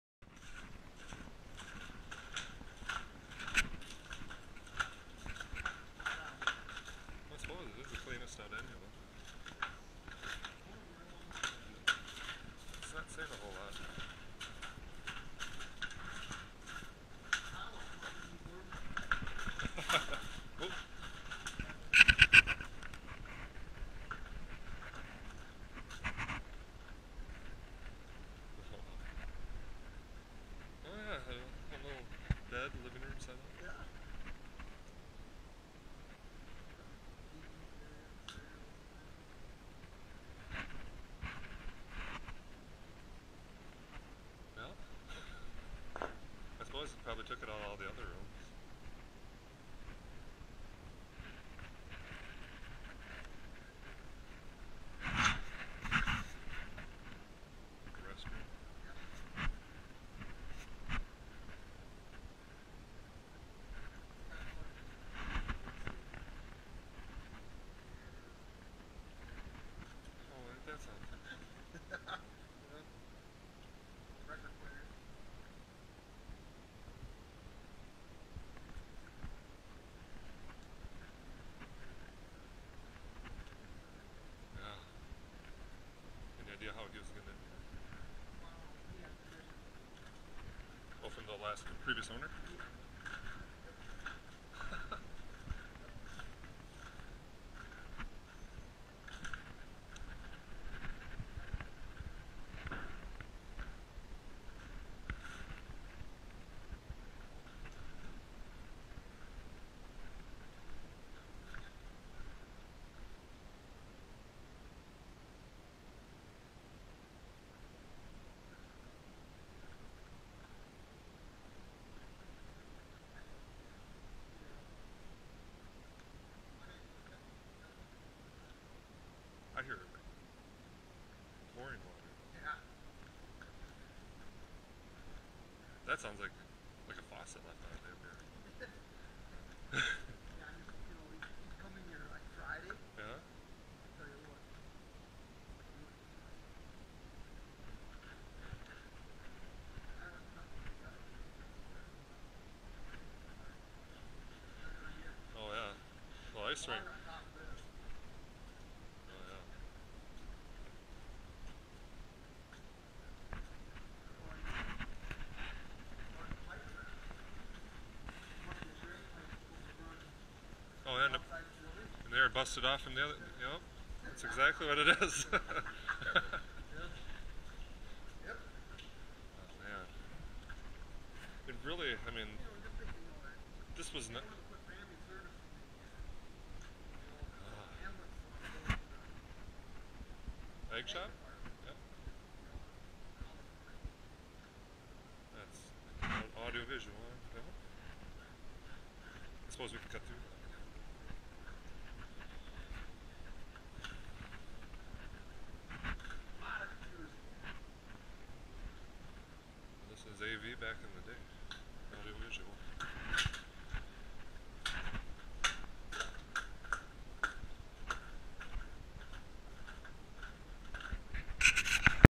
Abandoned Central School-1st Floor North End
1st Floor North end. Homeless person's getaway, Library and a leaking roof drainage line streaming into what was the Kid's Library, from what I remember. Courtesy of Chris Hollingsworth.